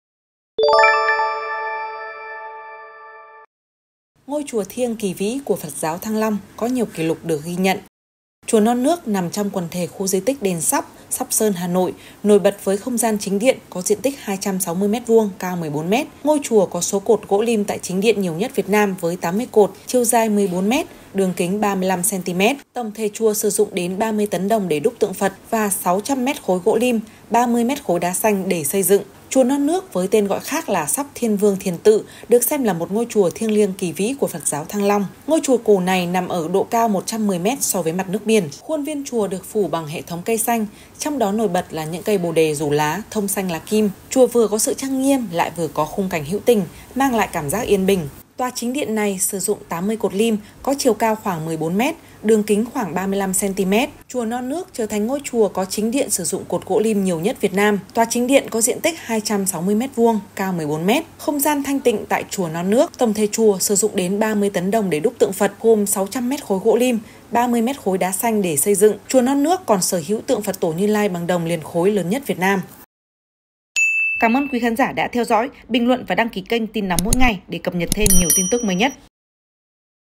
Ngôi chùa thiêng kỳ vĩ của Phật giáo Thăng Long có nhiều kỷ lục được ghi nhận
Chùa Non Nước nằm trong quần thể khu di tích Đền Sóc (Sóc Sơn - Hà Nội) nổi bật với không gian chính điện có diện tích 260m2, cao 14m. Ngôi chùa có số cột gỗ lim tại chính điện nhiều nhất Việt Nam với 80 cột, chiều dài 14m, đường kính 35cm. Tổng thể chùa sử dụng đến 30 tấn đồng để đúc tượng Phật và 600m3 gỗ lim, 30m3 đá xanh để xây dựng...